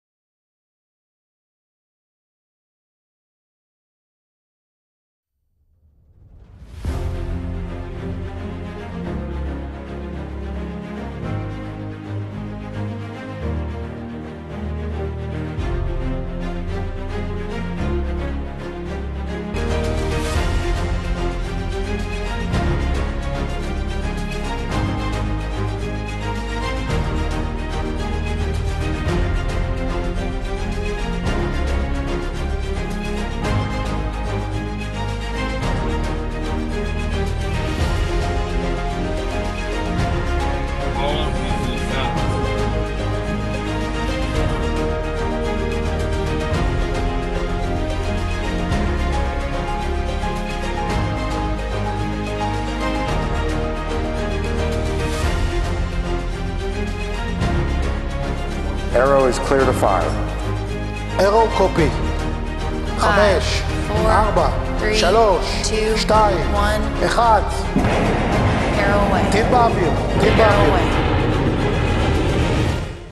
Israeli Ambassador to Washington, Ron Dermer, affixing a Mezuzah
The Israel Defense Ministry released a video of the
preparation and successful launch of the Israeli
Arrow 3 Missile.
In the video there is a short clip of the Israeli
Ambassador to Washington, Ron Dermer, affixing a
Mezuzah to the entrance of the test facility
of the Israeli Arrow 3 Missile.
Kal Hakavod on the missile launch and the Mezuzah.